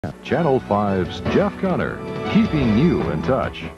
WOI (ABC) Station ID 1989 "Keeping You In Touch"
This is WOI's station ID from 1989
WOI is located in Ames, IA